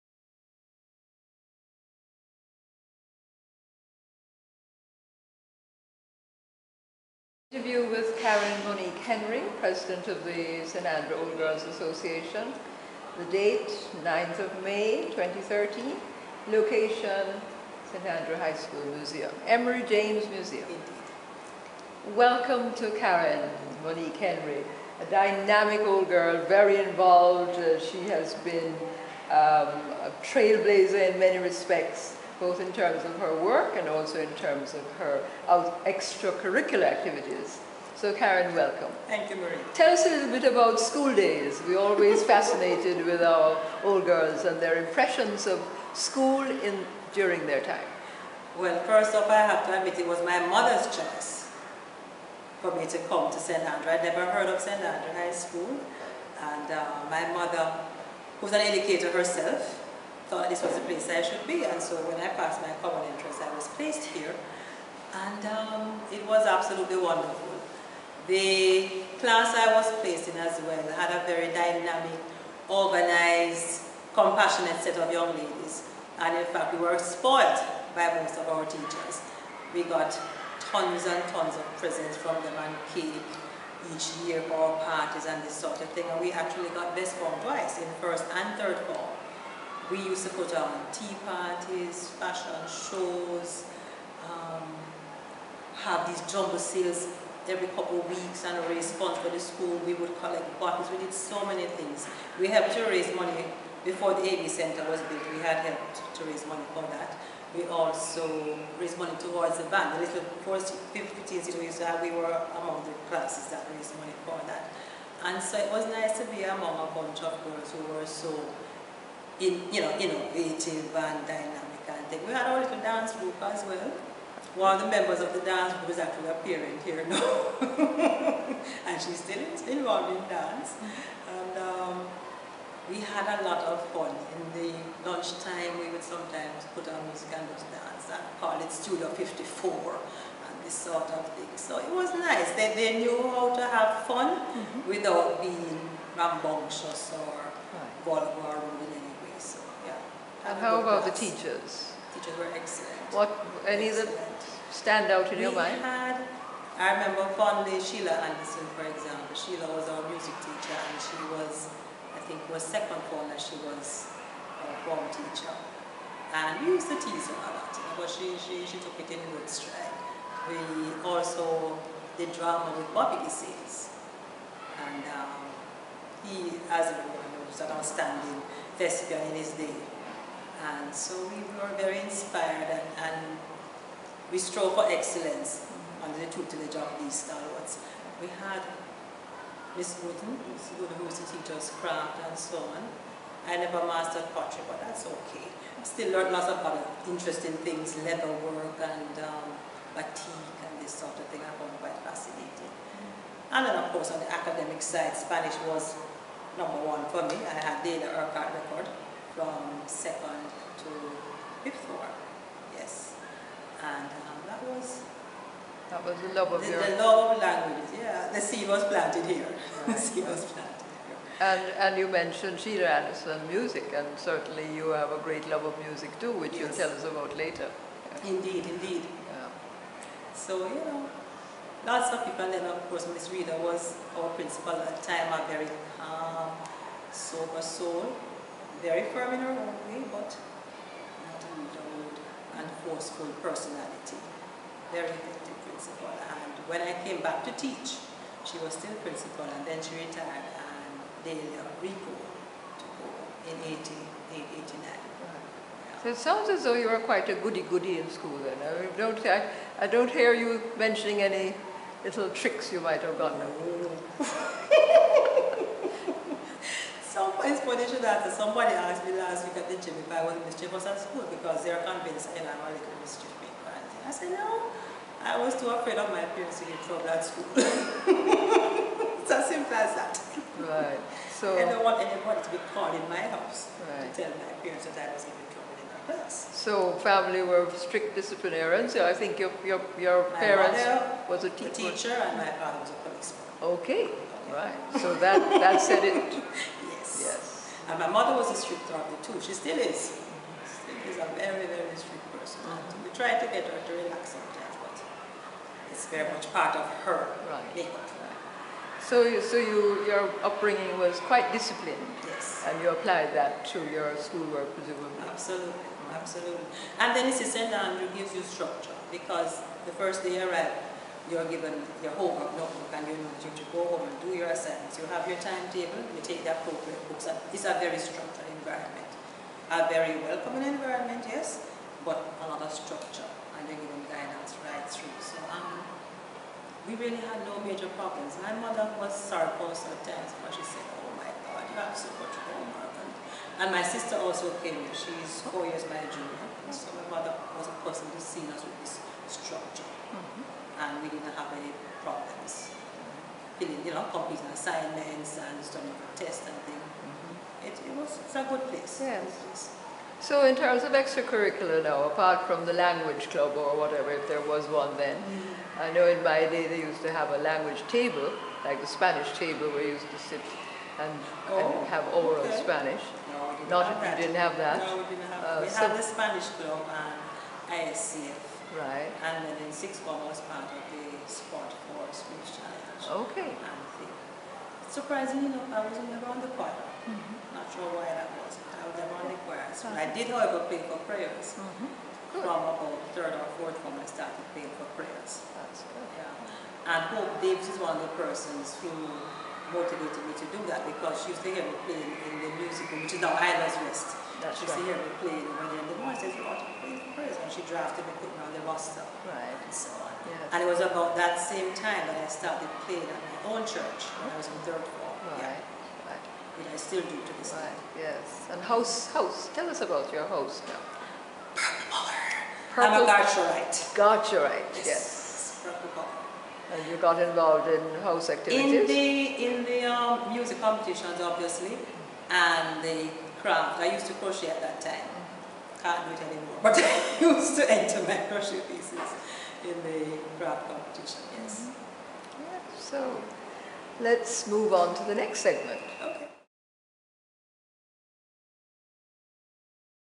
SAHS.360 - Pt 1, Karen Henry, President of the St. Andrew Old Girls' Association
Karen Henry, President of the St. Andrew Old Girls' Association (SAOGA); Translator at the World Bank (Spanish and French to English), Pianist and Church Choir Director.

Pt 1 - High School Life
Pt 2 - Karen's career path
Pt 3 - A fun segment

Interviewer: Marie (Hector) Clarke, SAHS trailblazer, Music Educator and retired Journalist

- EJM